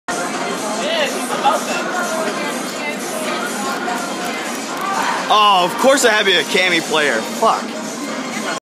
PAX East 2014 in a Furry Nutshell
this thing wouldn't leave me alone so i documented the abomination for shaming purposes before i ran away